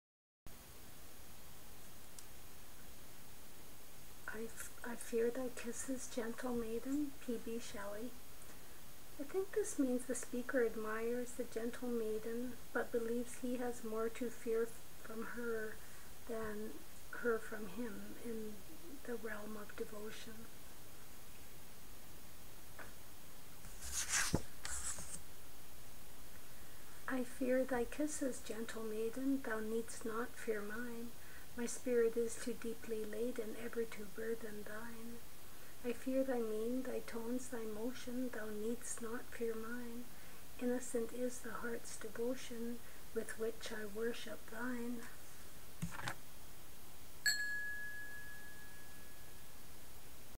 I fear thy kisses, gentle maiden - P. B. Shelley
176 I fear thy kisses, gentle maiden
The one-hundred and seventy-sixth poem in the collection.